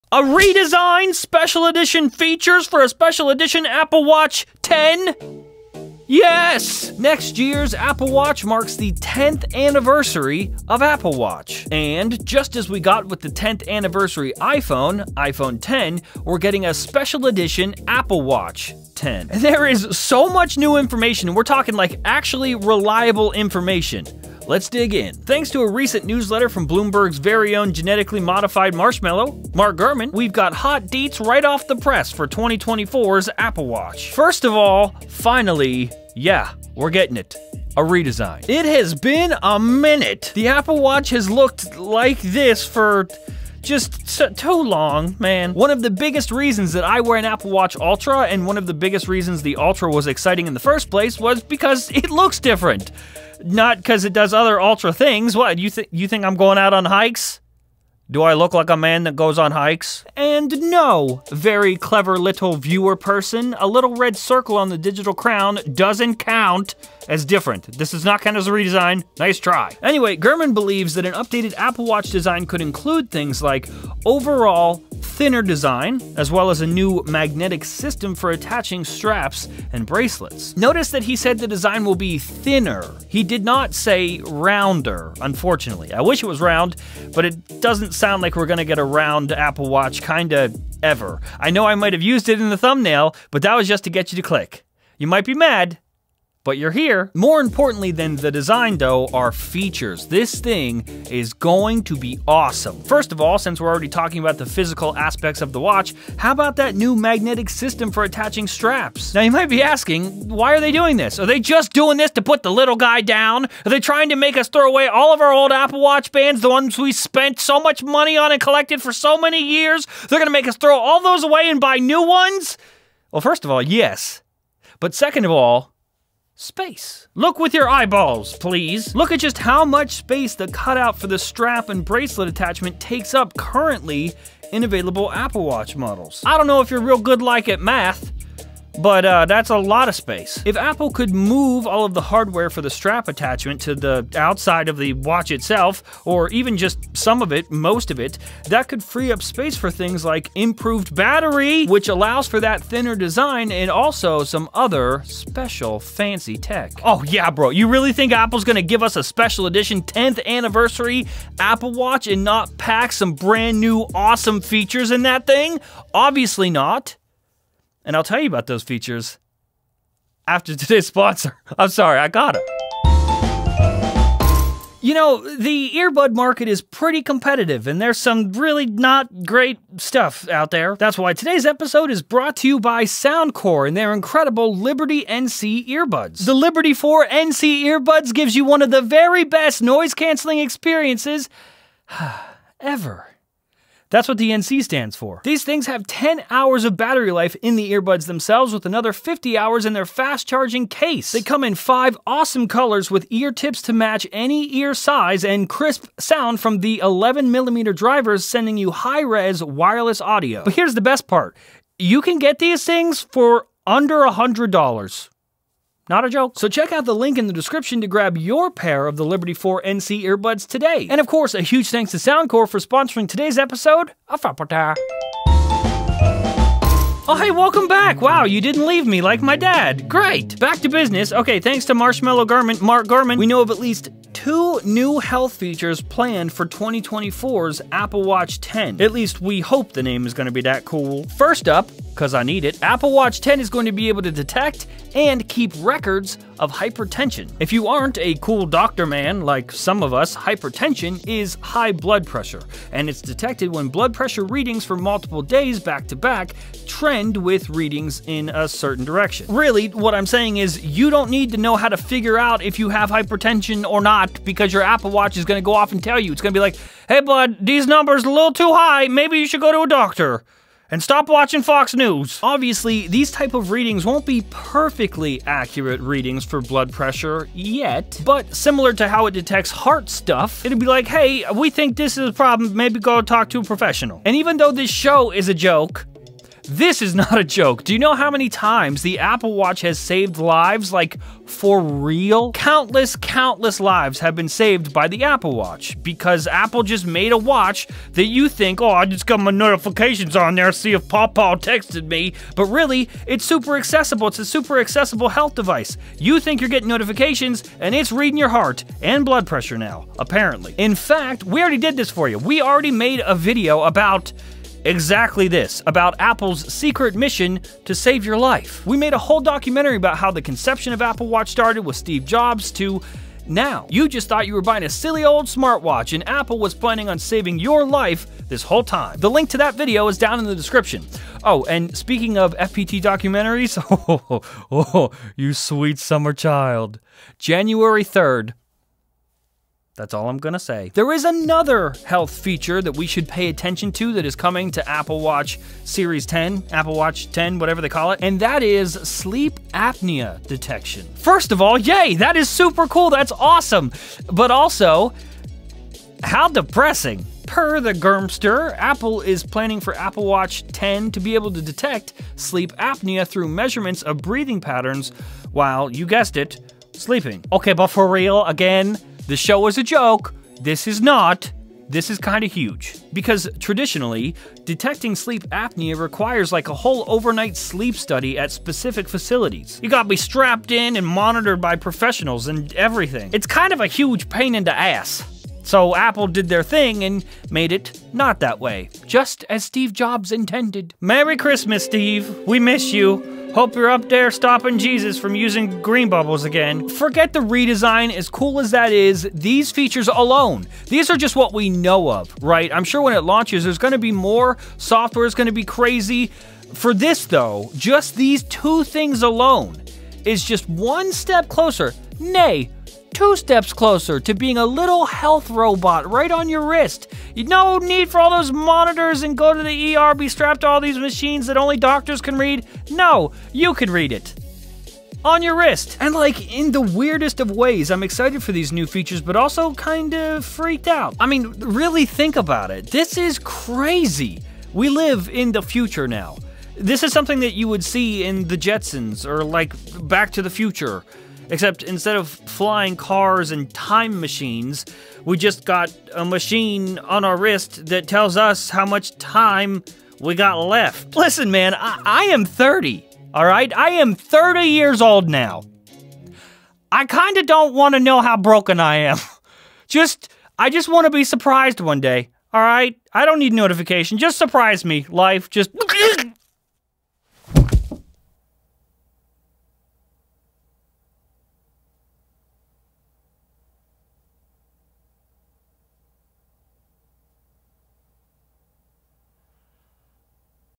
Apple Watch X - new DESIGN, new MAGNETIC BANDS, Blood Pressure, and more!
EP. 1249 - Apple Watch X - HERE YOU GO! new DESIGN, new MAGNETIC BANDS, Blood Pressure, and more! So many new Apple Watch Series X leaks!
Add soundcore Liberty 4 NC Earbuds to your cart now.

Chapters
0:00 New Apple Watch X leaks!
0:34 New Design
2:52 Sponsor
3:45 Blood pressure feature
5:56 Sleep apnea detectione nally